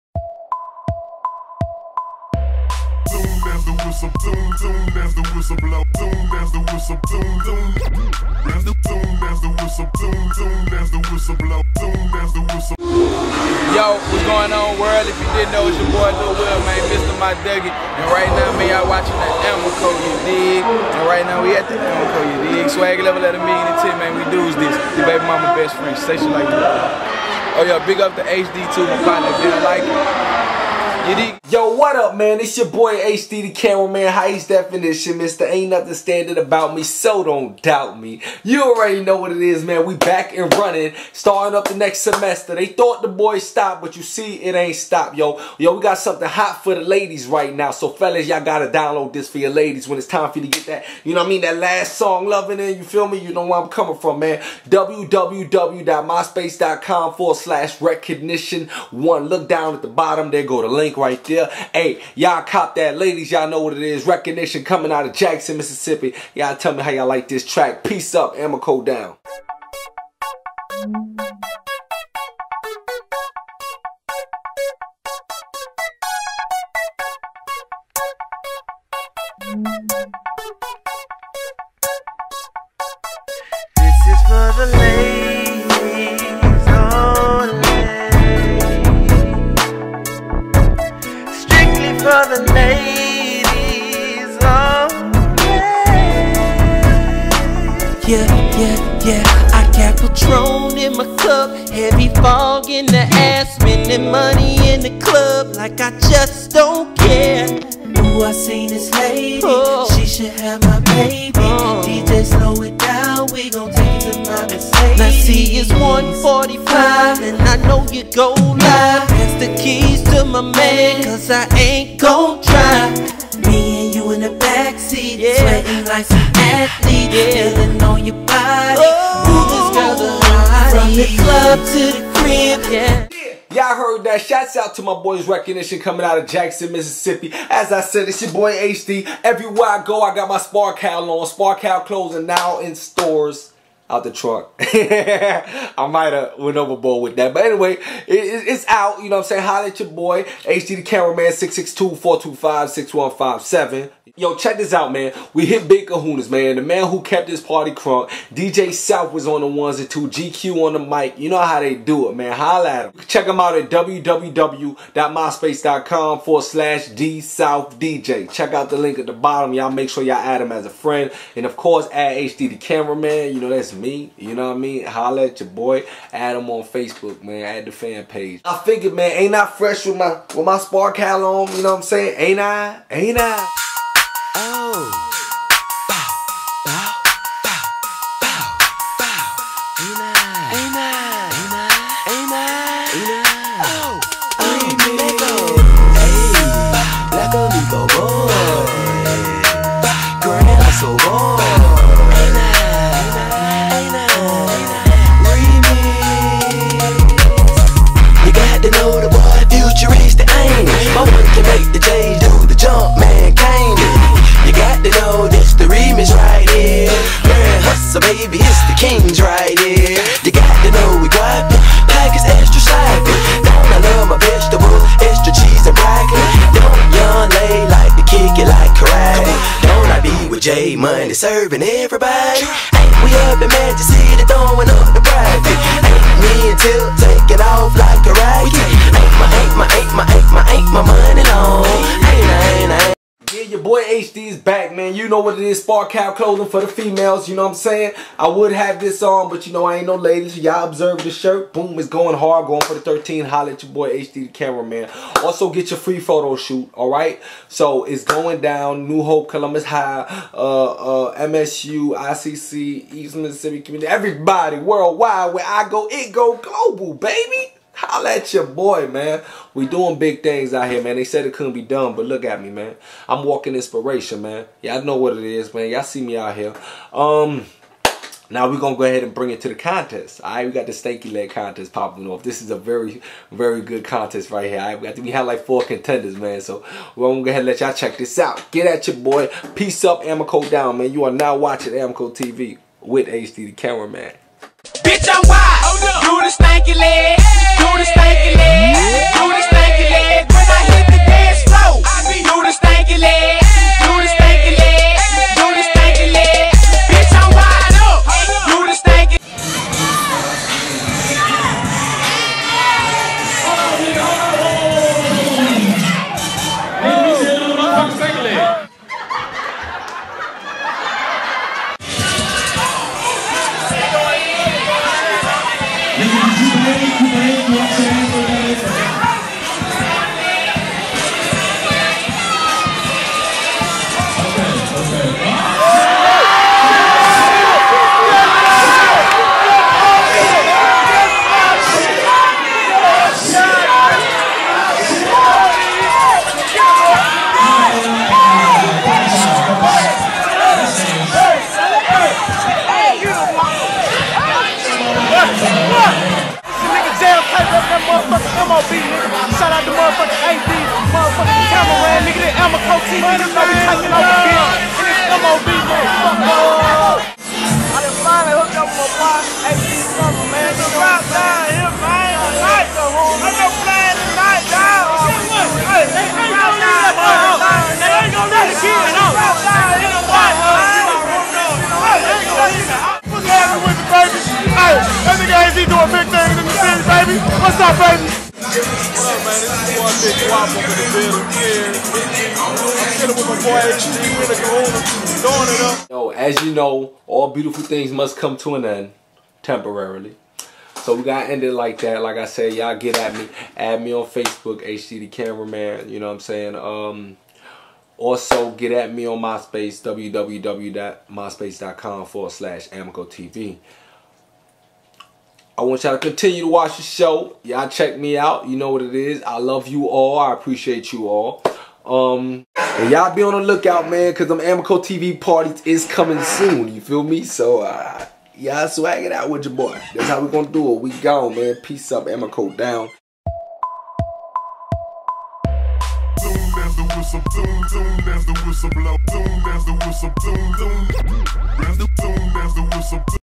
AMMACO TV HITS BIG KAHUNAS FOR X MAS
HD, Da Camera HITS COLUMBUS MS HOTTEST CLUB BIG KAHUNAS.  Join group AMMACO T.V ON FACEBOOK & LETS DEVELOP THESE MEMORIES TOGETHER.

JOIN GROUP AMMACO T.V ON FACEBOOK

ADD ME ON MYSPACE.COM/AMMACOTV